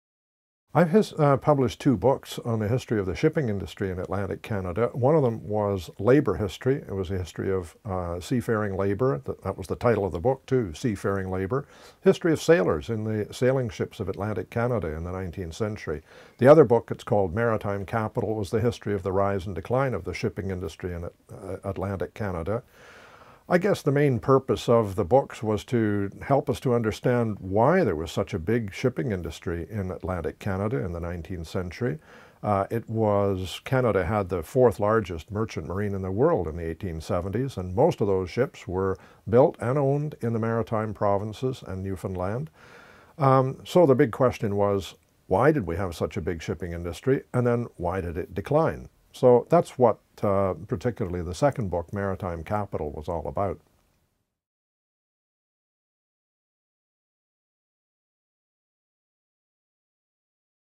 Dr. Eric Sager Question 2 - Key themes in the history of maritime trade.
What are key themes in the field of history of maritime trade in the 19th century?